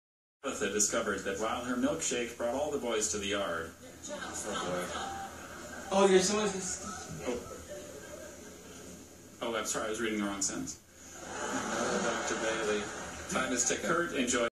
Scripps National Spelling Bee Blooper
Jacques Bailly reads the wrong sentence